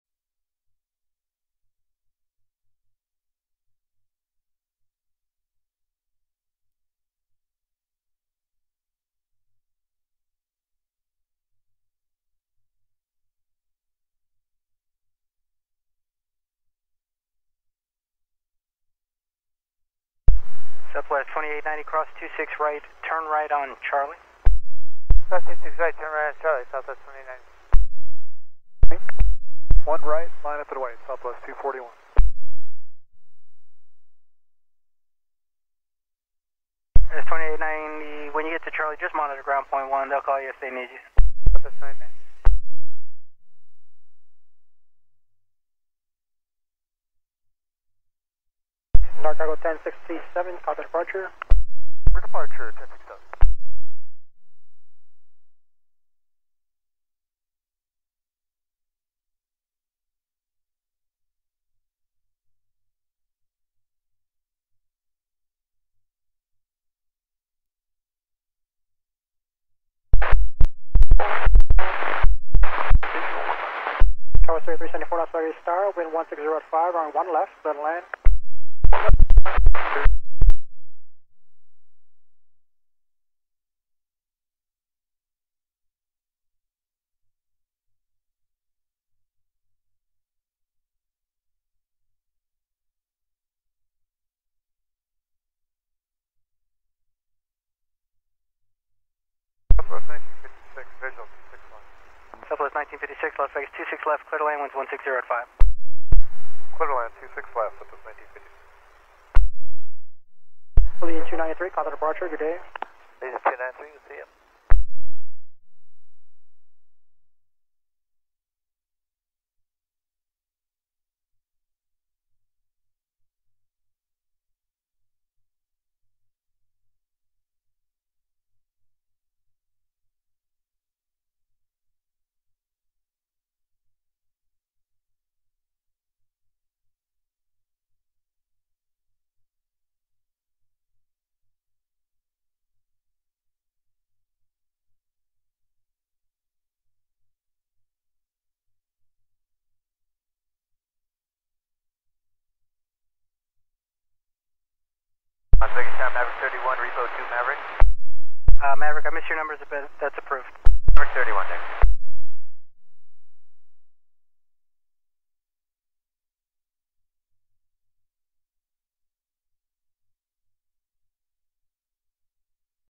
Airbus A320 landing gear deployment (Frontier Airlines)
Captured at Harry Reid International Airport on August 19, 2022.